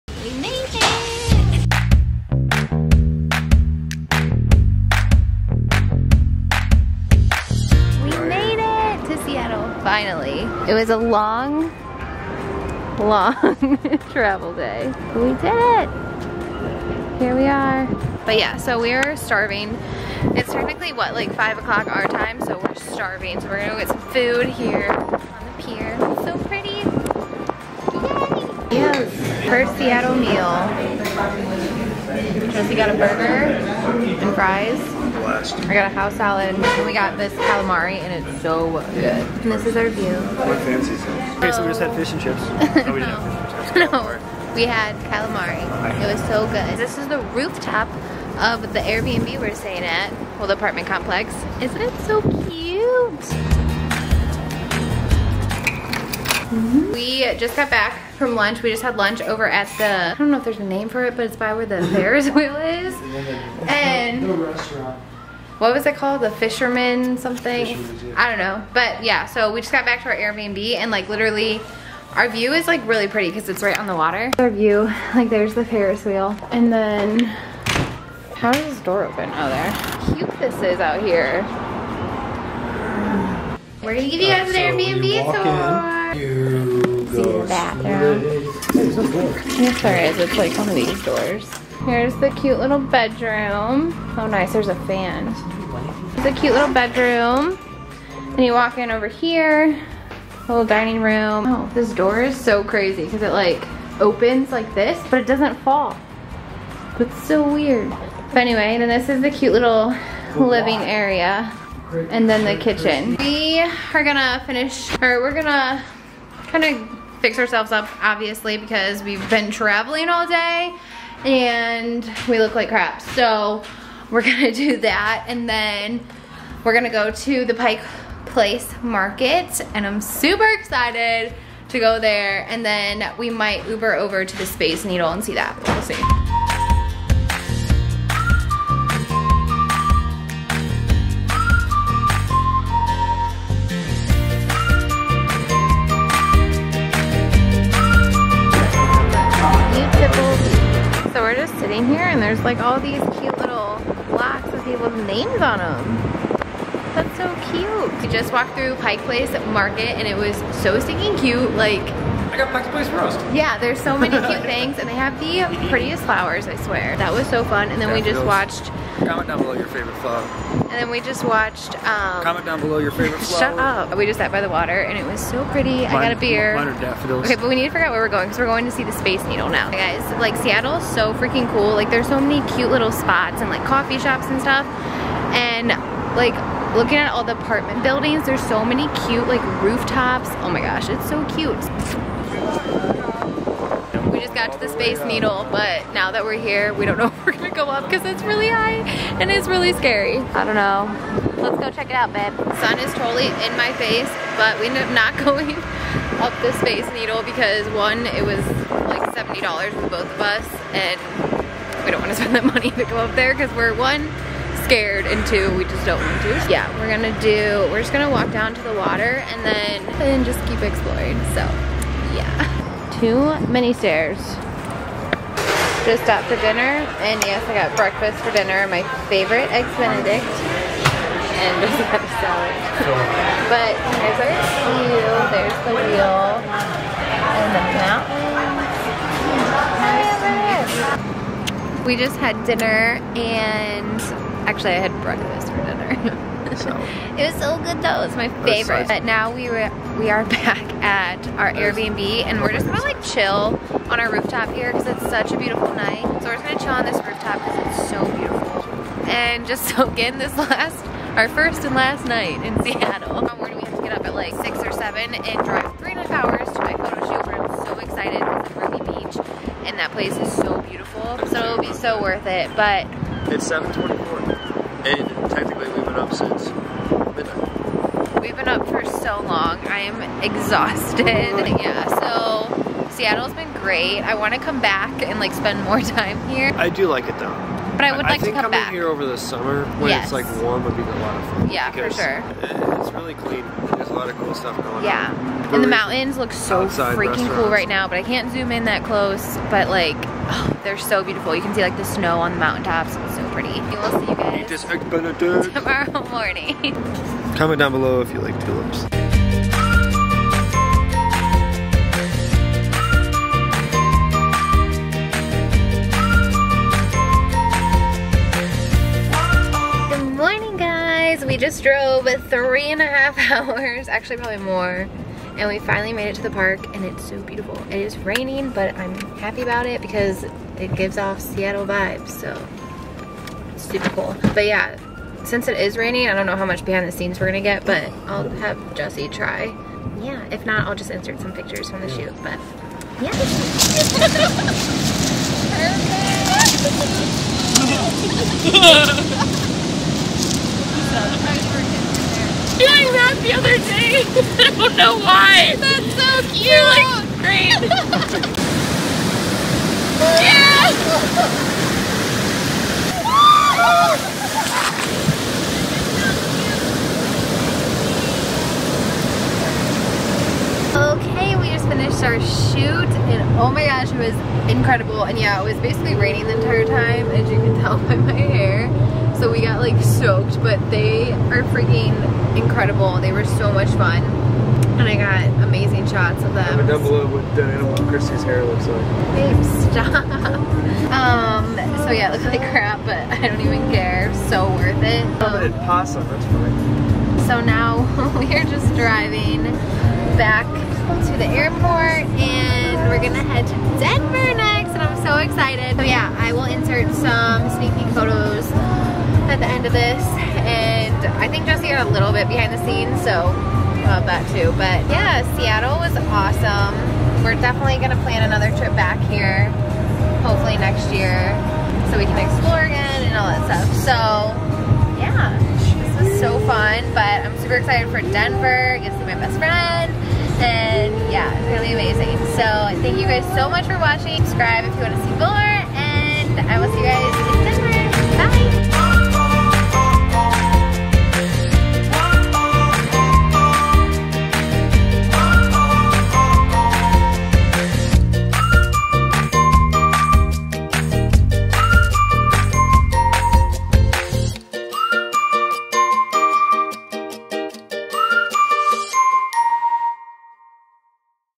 SEATTLE VLOG | pike place market, space needle, & more!
Traveling to Seattle for the first time! We went to Pike Place Market, the space needle and just explored! Enjoy!

// Video Editor & Equipment:
Final Cut Pro
Vlog camera:

xo ♡
Christy